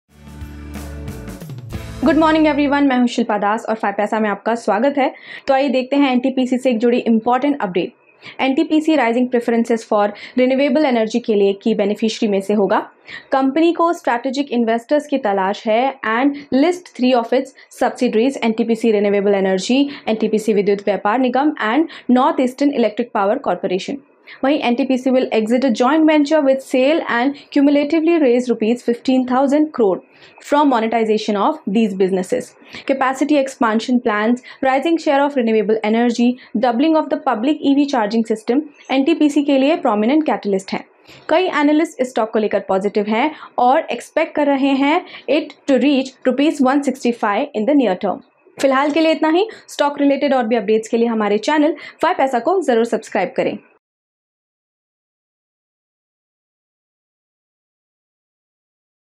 NTPC: IPOs of renewable business, other subsidiaries to drive stock | NTPC Share Latest Updates
NTPC will be among the key beneficiaries of rising preference for renewable energy. Company is looking for strategic investors and list 3 of its subsidiaries - NTPC Renewable Energy, NTPC Vidyut Vyapar Nigam and North Eastern Electric Power Corporation.

NTPC will exit a joint venture with SAIL and cumulatively raise Rs. 15,000 crore from monetization of these businesses. Capacity expansion plans, rising share of renewable energy, doubling of the public EV charging system are prominent catalysts for NTPC.

Most analysts are positive on the stock and expect it to reach Rs. 165 in the near term.

Suggested Videos